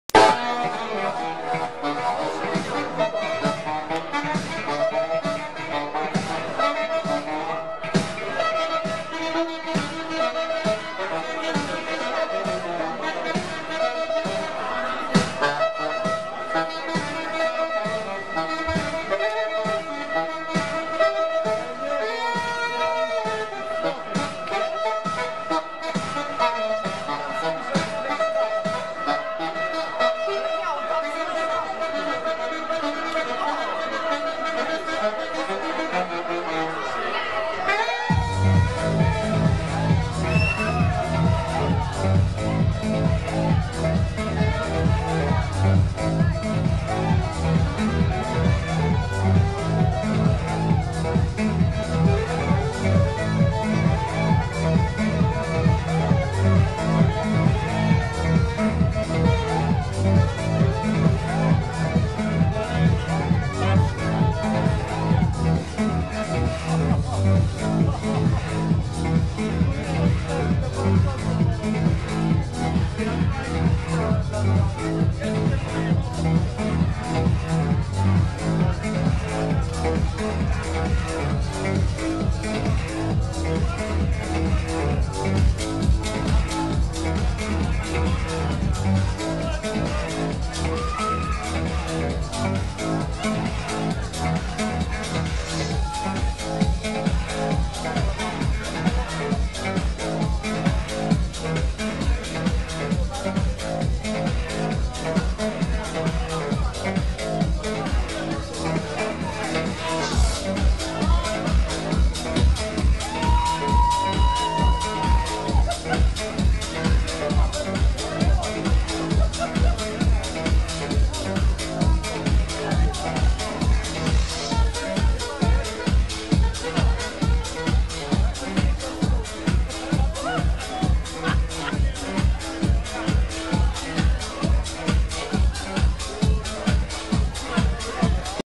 Filip Domarski live saxophone with Beats of Silky Style!
Beats of Silky Style na Szczepana,
Filip Domarski - saxophone.
Estrada Caffe, 26 grudnia 2012 r., Rzeszów.